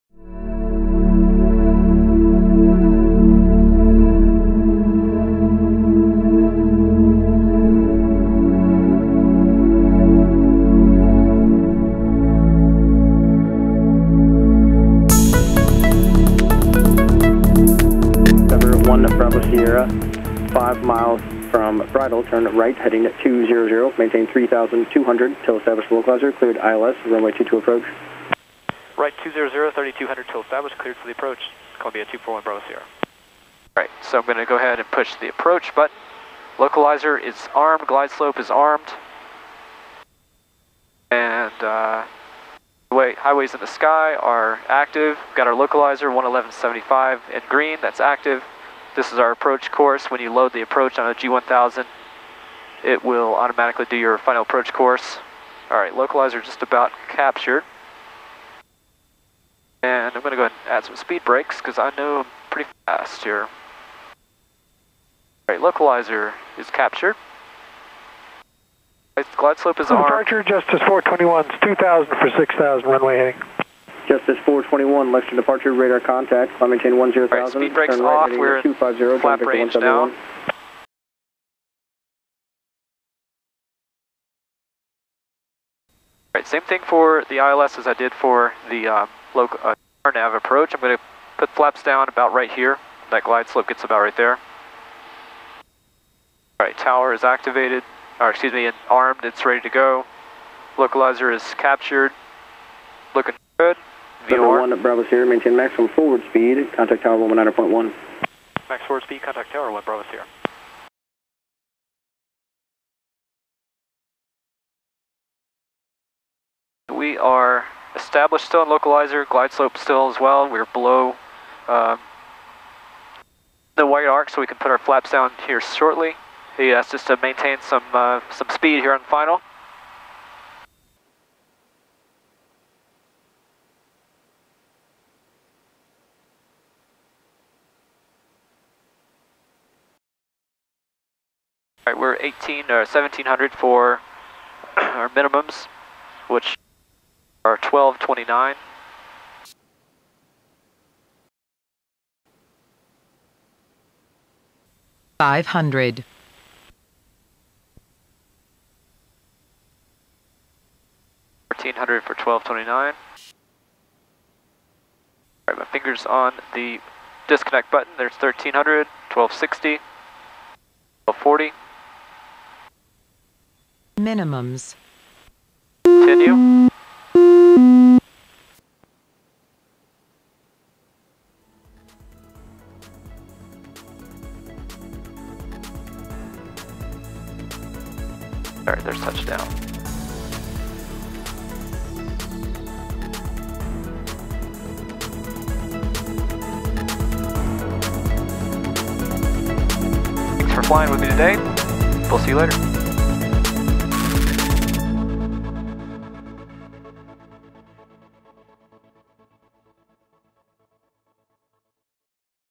AirMart - Flying an ILS Approach with the Garmin G1000/GFC700
Fly along with AirMart's Demonstration Pilot,  Matt Gabbard, as he demonstrates the capability of the Garmin GFC700 automatic flight control system. He's flying the ILS to runway 22 at Lexington, KY (KLEX) in a 2008 Cessna 350 equipped with the Garmin G1000 with Synthetic Vision Technology (SVT).

AirMart
4144 Aviator Rd, Suite 5D, Lexington, KY 40510 USA